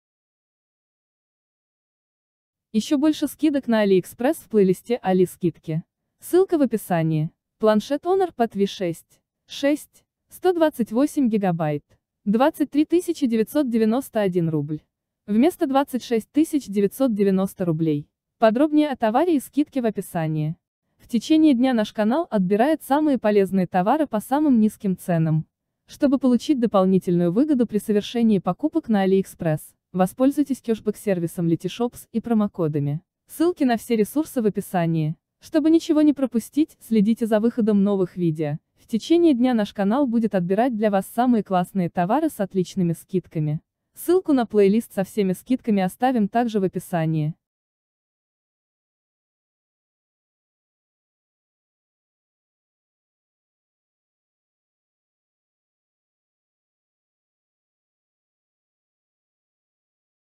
Планшет Honor Pad V6 6\128ГБ | скидки на Али
Планшет Honor Pad V6 6\128ГБ
На TMALL самая низкая цена за всё время на этот девайс, скидка будет действовать с 07.12 по 12.12. Для тех кто хочет купить рядом с домом есть вариант 24990р в СИТИЛИНК по промокоду TABSALE, что тоже очень выгодно, особенно с бонусами\кэшбэками, халвой той же например(по халве кэшбэк 1500р и цена будет выгоднее чем на TMALL).
В остальных магазинах 26990р или 29990р.
Экран: 10.4" FHD+ 2000x1200 IPS sRGB 100% Яркость 550 кд/м2
Процессор: Kirin 985
1x Cortex-A76 2.58 ГГц 3xCortex-A76 2.40 ГГц 4xCortex-A55 1.84 ГГц
7 нм
Видеоускоритель: Mali G77 MP8
Память: 6\128ГБ
Камера: 13 Мп ƒ/1.8
Фронтальная камера: 8 Мп f/2.2
Аккумулятор: 7250 мАч
Сеть: Wi-Fi a/b/g/n/ac/ax DualBand, Bluetooth 5.1 (BLE, SBC, AAC, LDAC HD, HWA Audio)
Зарядка: USB 3.1 Gen 1 Type-C 22 Вт
Размеры: 245.2 мм*154.96 мм*7.8 мм
Вес:475г
Аудио: 4 динамика Harman / Kardo

Кешбек сервис LetyShop (скидка до 80% в более 1500 магазинов, в том числе Aliexpress.)